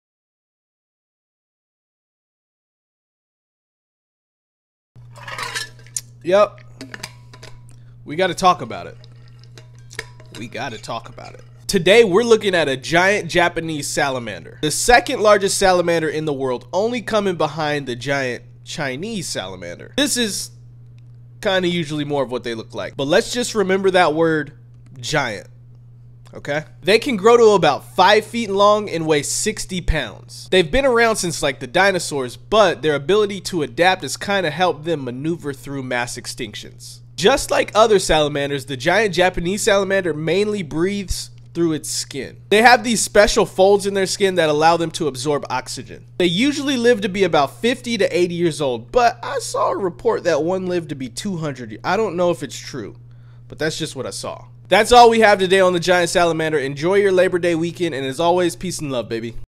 The Giant Japanese Salamander!
Giant Japanese Salamander

Japanese giant salamanders breathe primarily through their skin. The smooth skin acts as a respiratory surface for gas exchange, where oxygen enters the body and carbon dioxide is released.

They do also have lungs but they’re not really used as much as you’d think.

Could you guess what their biggest threat is.

That’s right you guessed it, humans.
We gotta do better man
Not too long ago these creatures were almost extinct because ya know .. we eat them & habitat loss.

But back in the 50s Japan designated this animal as a special natural monument making it illegal to hunt them & they’ve kind of been making a comeback.

These amphibians grow to about 5 feet long and 50-60 pounds. (152cm, 27.2kg)

They live to be around 50-80 years old.

There’s even reports of a 200 year old giant salamander.